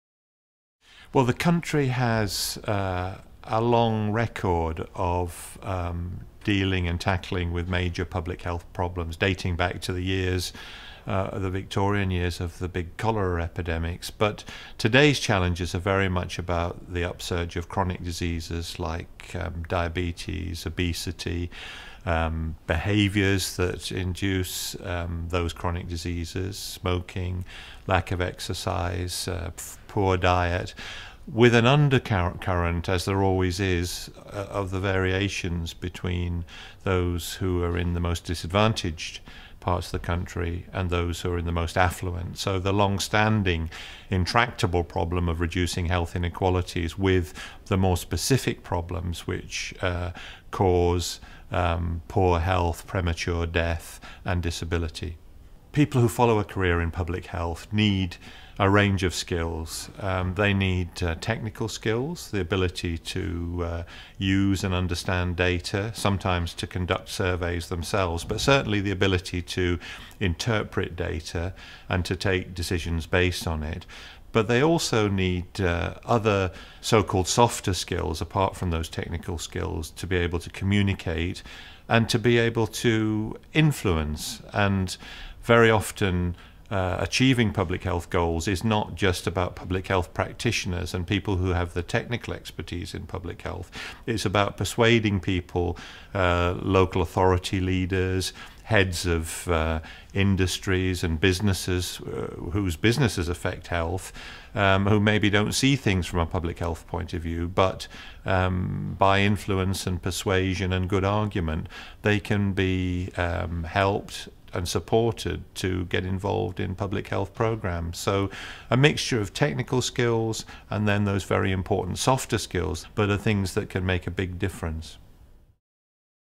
Public Health - Sir Liam Donaldson
Find out more about public health isues by watching the interview with Newcastle University's Chancellor, Sir Liam Donaldson.